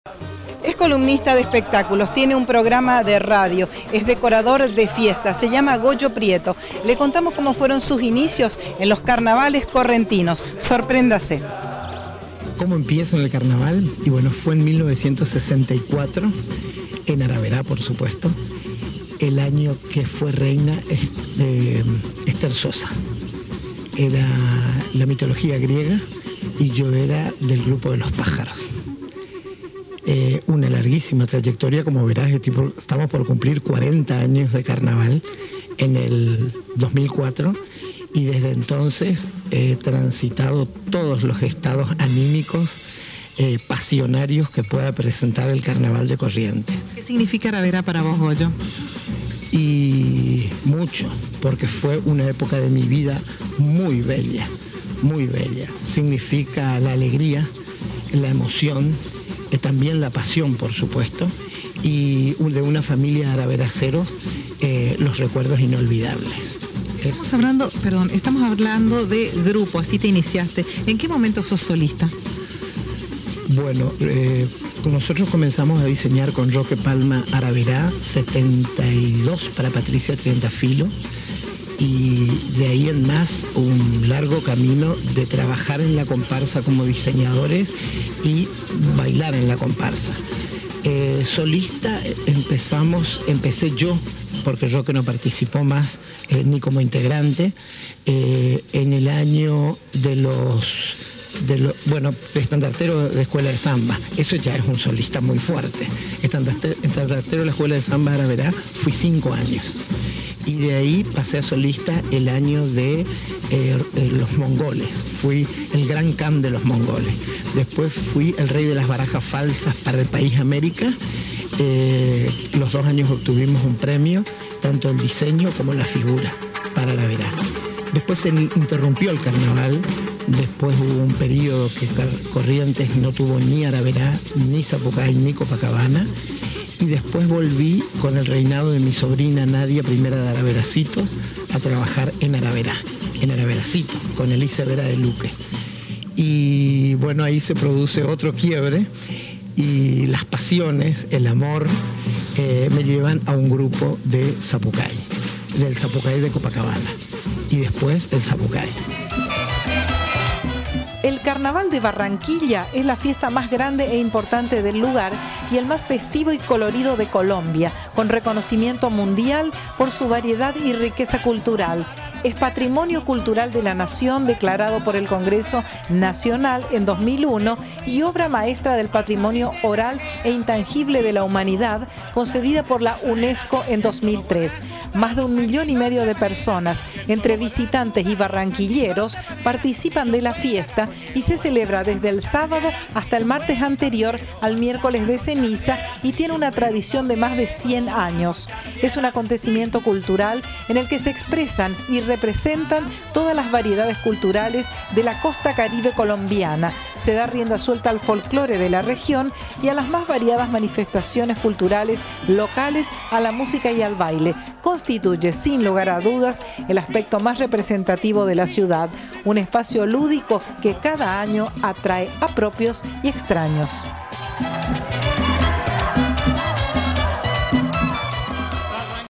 Goyo Prieto
Sus inicios en el carnaval como comparsero . . .